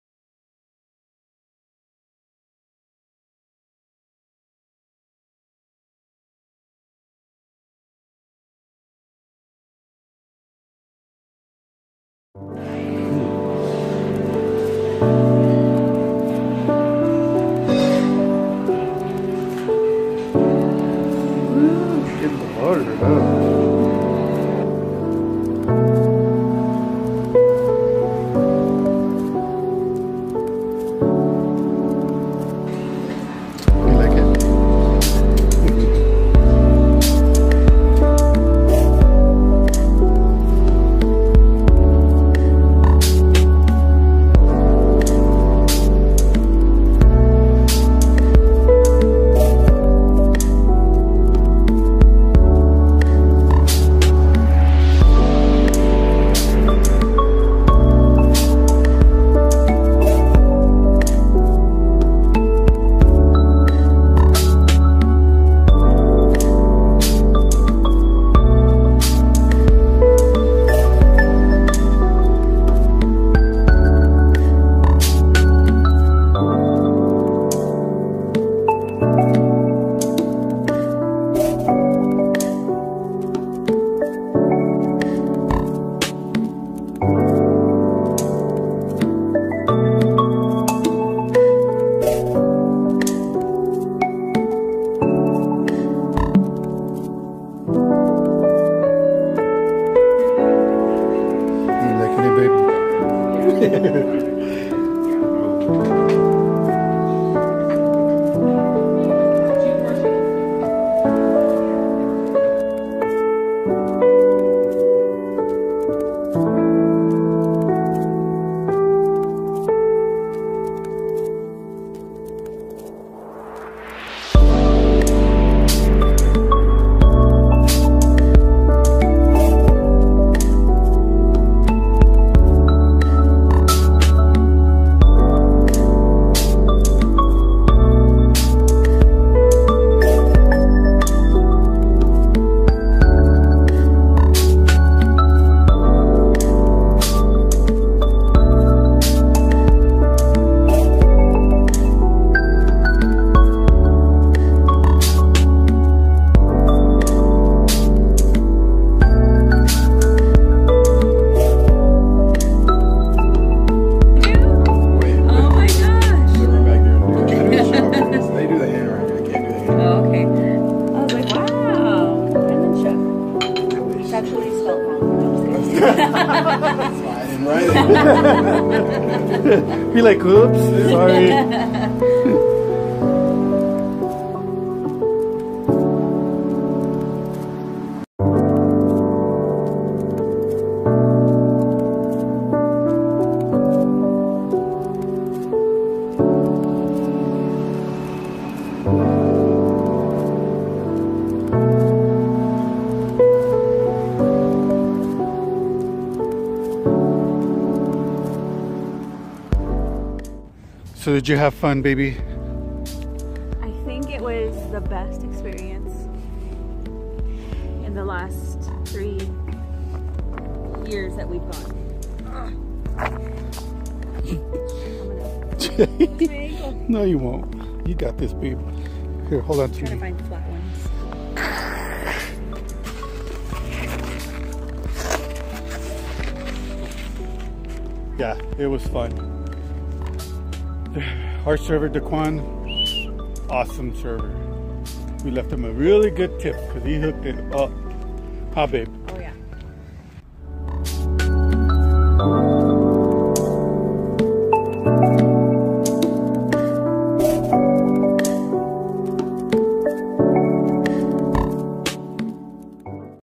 We celebrated our 16 year wedding anniversary at the Melting Pot | The Villa Family Vlogs
We started a new tradition since moving back to Albuquerque which is eating dinner at the Melting Pot.

We love it here and we've never been disappointed.

Thank you Melting Pot for having us and our badass server D'quan.

➡️➡️HELP US REACH 1K SUB⬅️⬅️

Filmed with my Sprint Samsung Note 5

Mic: Rode Microphones VideoMicro Link to purchse

Tripod: PolarPro Trippler Tripod Link to purchase